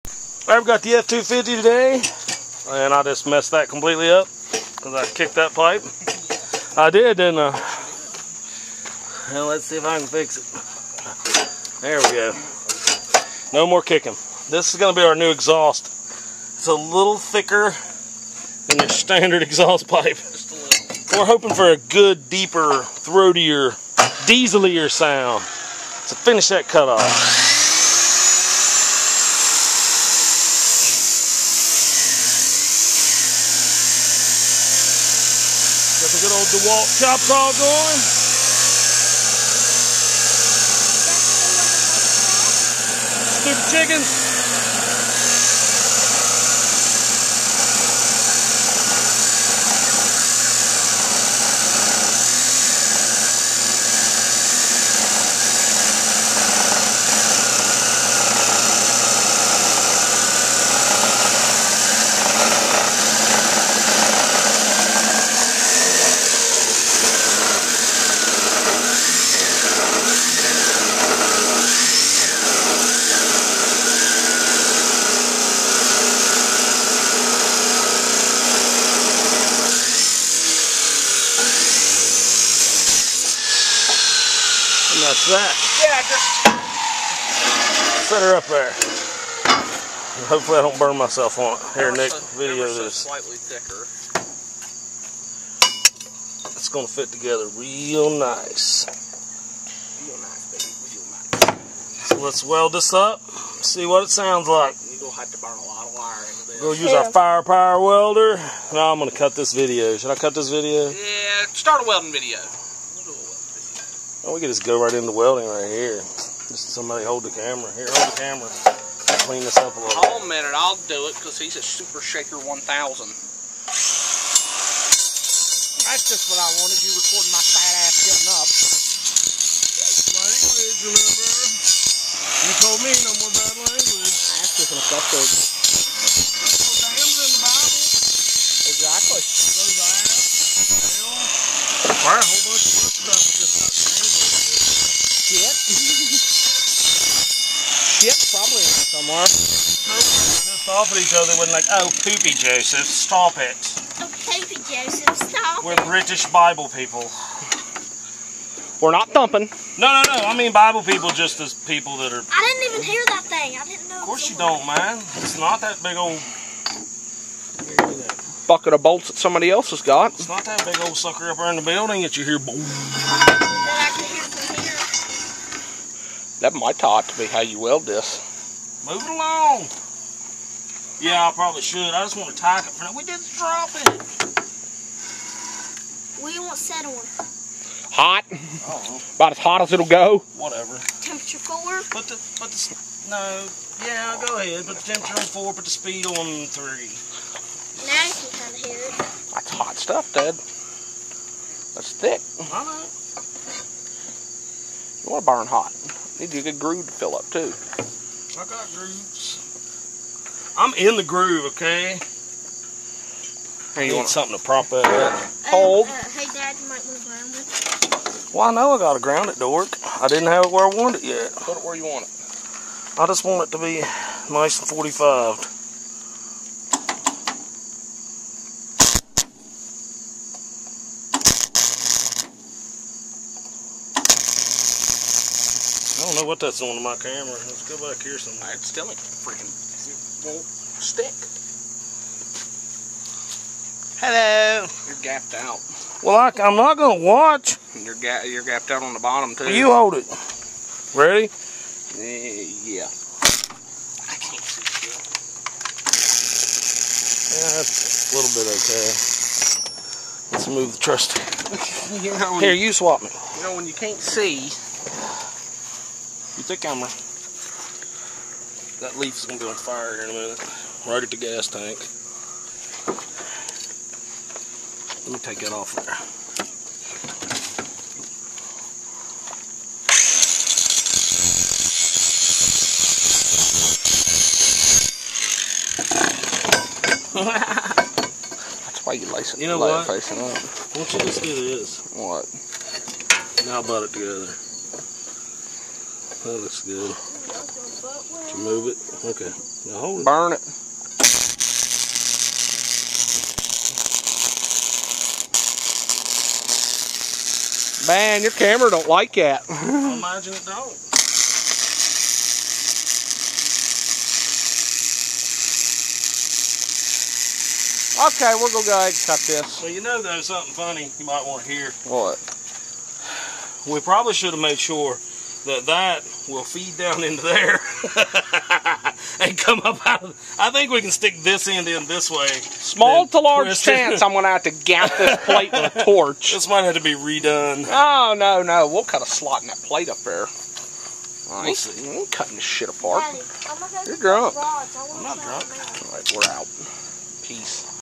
F250 new exhaust
Who would not want a 2 inch triple strength gas line for an exhaust? We will do another vid on how good it sounds